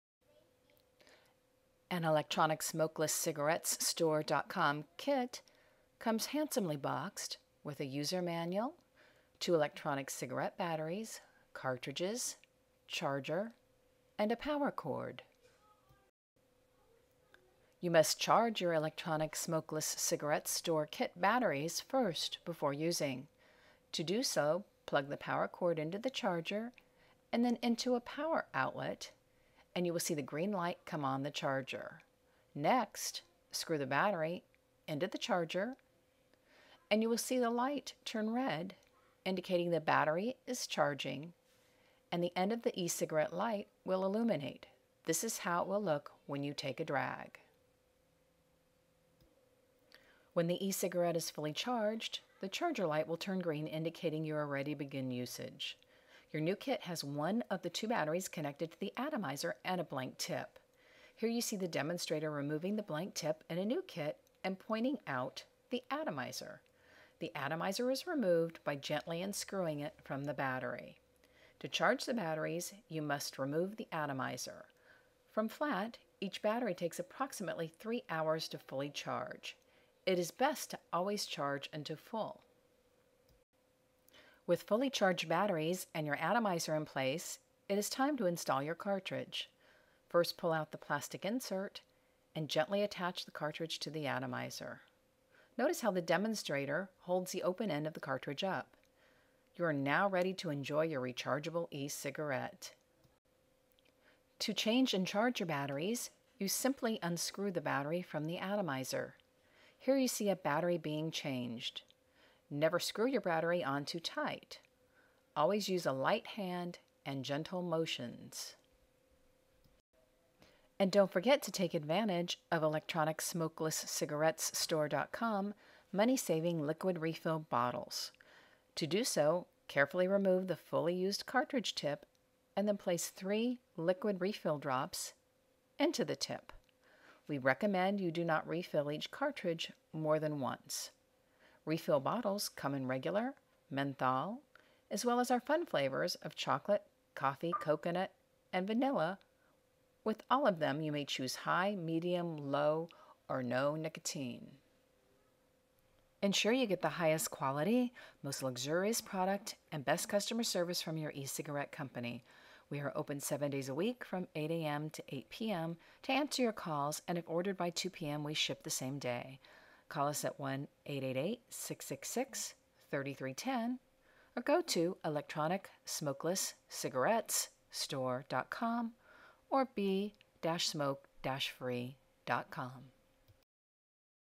How Electronic Cigarettes Work
Learn how an Electronic Cigarette Works. Be Smoke Free and enjoy the freedom to smoke anywhere with an E Cigarette.